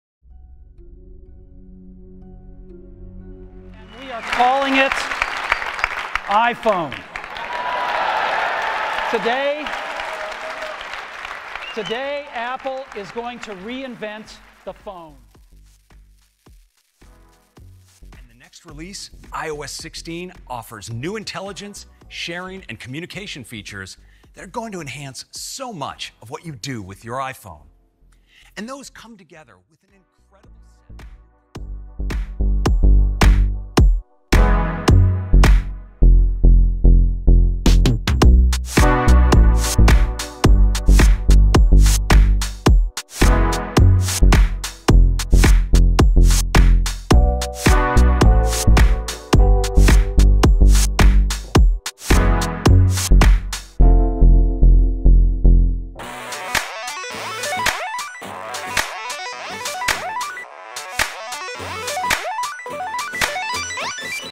iOS 16 on the 1st iPhone (Concept)
Roughly 16 years ago, Apple unveiled the first iPhone, running iPhone OS 1. Fast forward 16 years and iOS 16 is released. iOS 16 is (obviously) much more capable and powerful than iPhone OS 1, from basic features to advanced customization, privacy, intelligence features, and so much more. So, theoretically, how would the first iPhone look like if it ran iOS 16?

Everything (except for the icons and wallpapers and the part where Craig goes down the elevator) was designed by me in Figma and animated in Rive and After Effects.
The skeuomorphic icons are made by the extremely talented @THEVOLKANOGEDAY and @NDdoesstuff. Give them a follow on Twitter!

Also, I'd love to hear from you in the comments!

I remade the MacWorld 2007 banner, and you can find it
All wallpapers are from Apple. One or two are from Apple's website, but the majority of them can be found